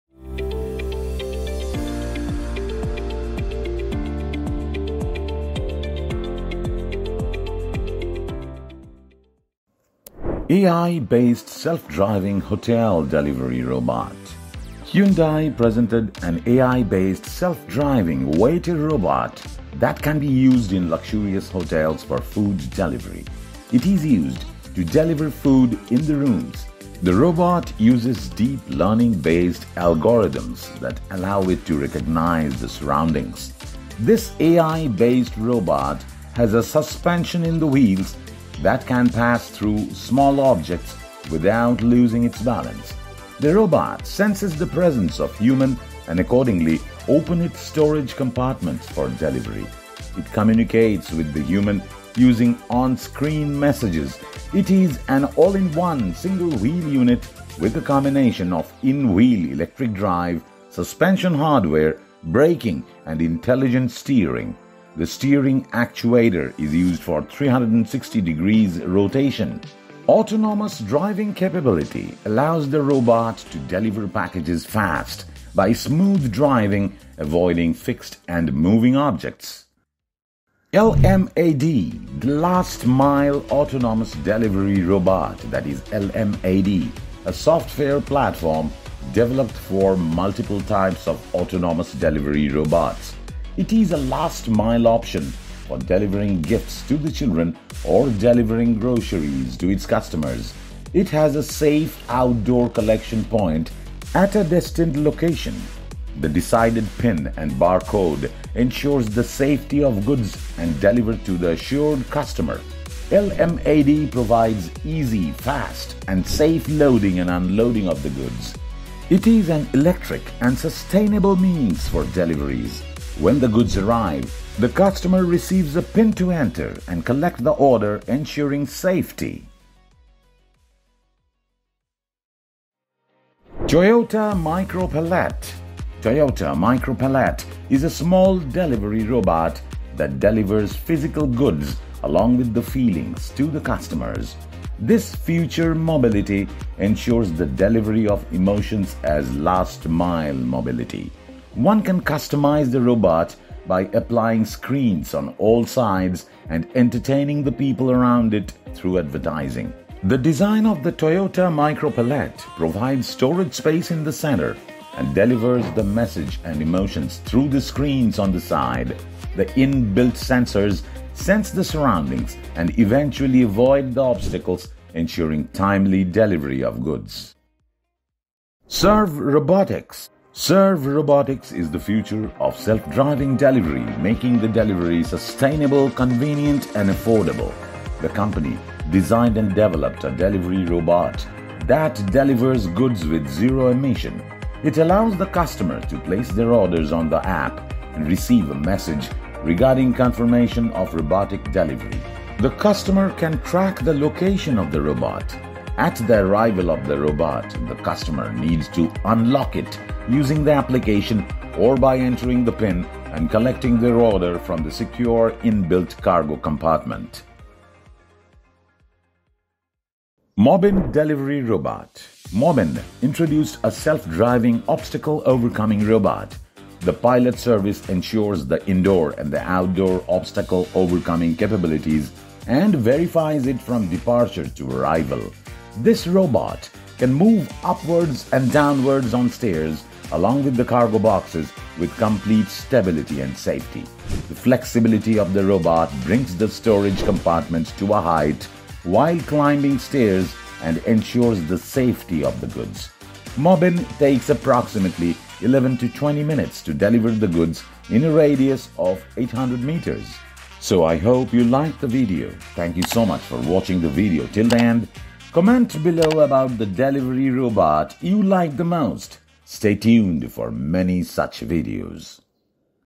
5 Mind-Blowing Self Driving (Autonomous) Delivery Robots | ▶2
5 Mind-Blowing Self-Driving (Autonomous) Delivery Robots | ▶2

AI-based Self-Driving Hotel Delivery Robot by Hyundai Motor Group
LMAD
Toyota Micro Palette
Serve Robotics
Mobinn Delivery Robot